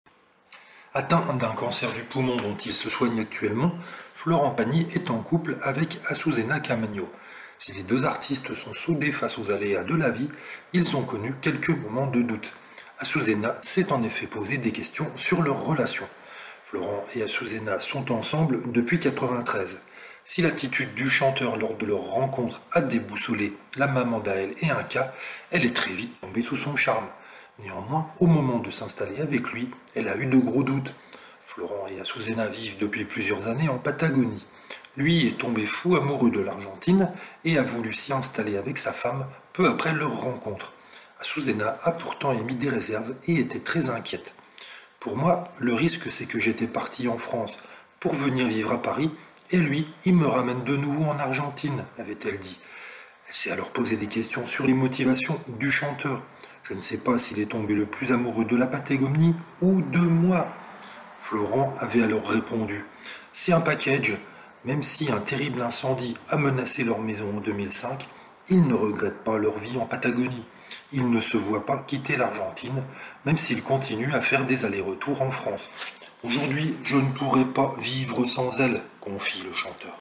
Florent Pagny : les gros doutes de sa femme Azucena
Atteint d'un cancer, Florent Pagny a adopté la Patagonie